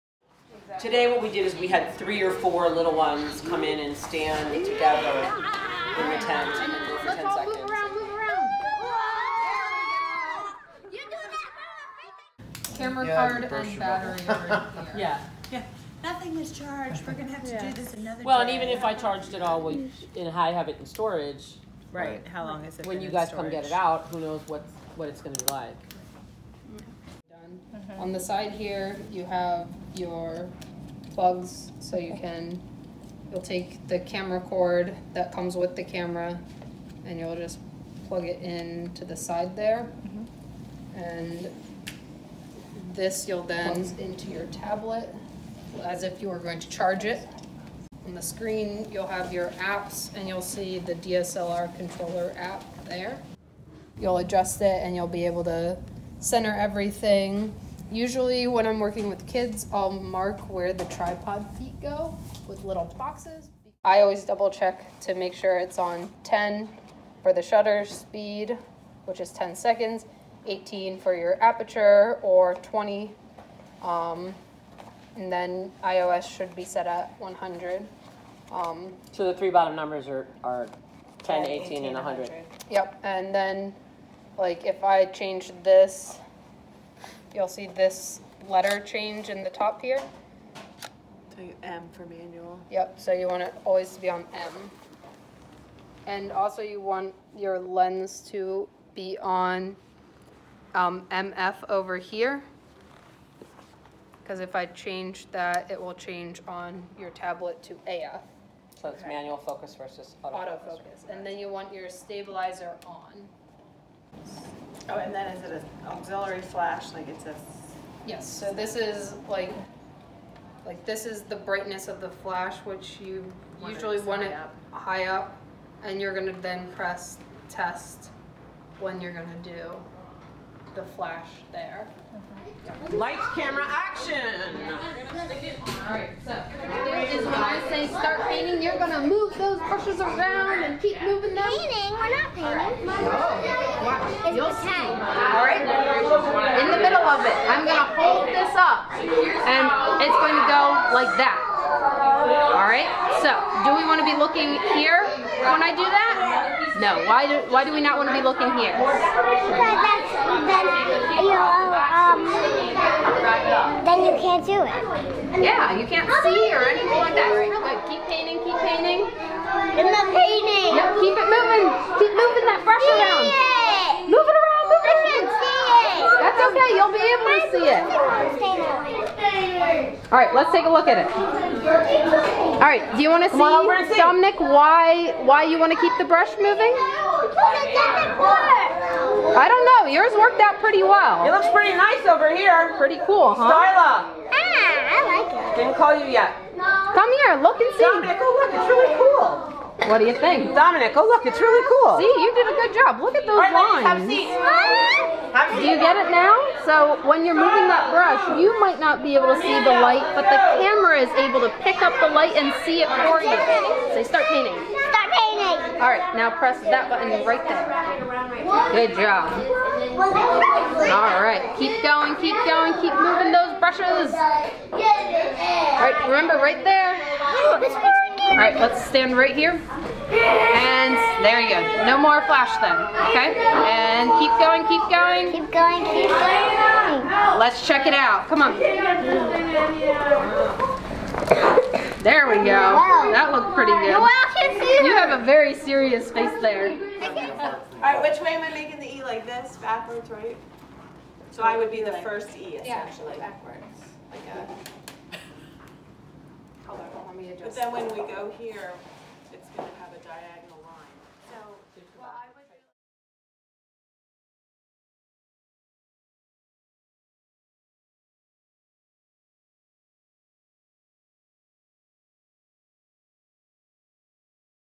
Light Painting for EEE teachers
Anne Arundel County Public Schools' EEE Program is designed to spark creativity and teaches students to approach problems with an open mind. Thanks to educator Betsy Brininger and YA roster artist FutureMakers, teachers now have the resources to integrate light painting into their classes. The EEE department's digital camera is set to capture long exposures with the help of a tablet installed with DSLR Controller We can't wait to see what subjects students will explore through light painting!

FutureMakers can help you bring this and other creative technologies into your child's classroom